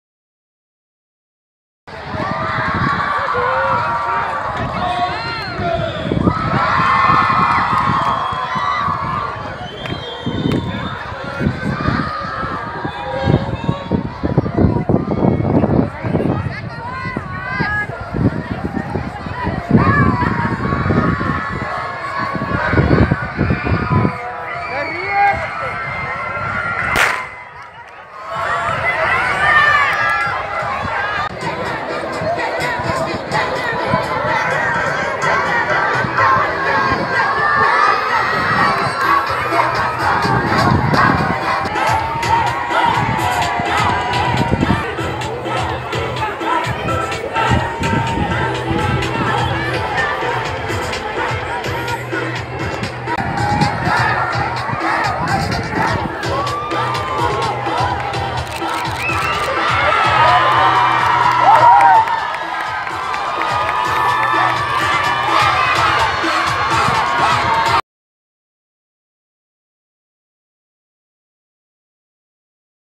Pret en plesier by atletiek
Hoërskool Punt se jaarlikse Interhuis atletiekbyeenkoms het vandag plaasgevind. . Terwyl ywerige atlete met erns deelgeneem het aan hul onderskeie items, het 'n algemene gees van pret en plesier onder die leerders geheers.

Video: Tersia Marais, Mossel Bay Advertiser